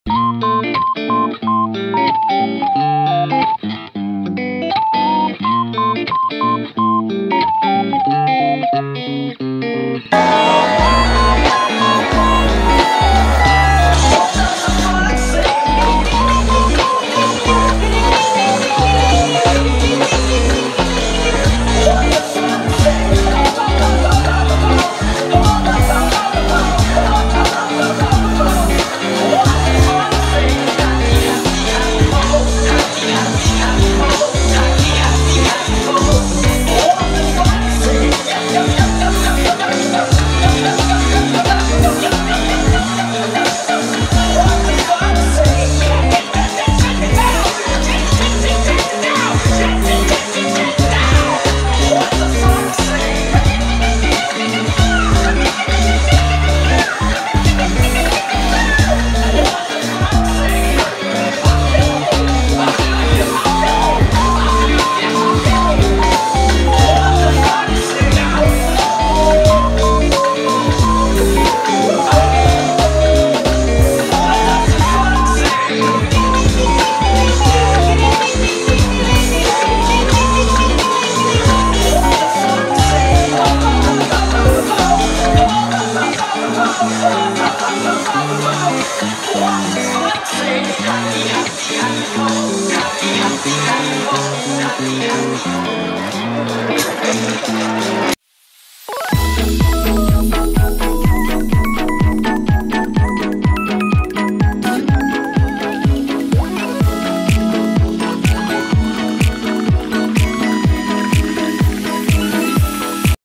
20230414【きつねダンス】竹市琴美 大野未侑 辻菜穂子
2023年4月14日

エスコンフィールド北海道の西武戦
竹市琴美 大野未侑 辻菜穂子のきつねダンス
きつねダンスパフォーマンス

使っているカメラ
COOLPIX P950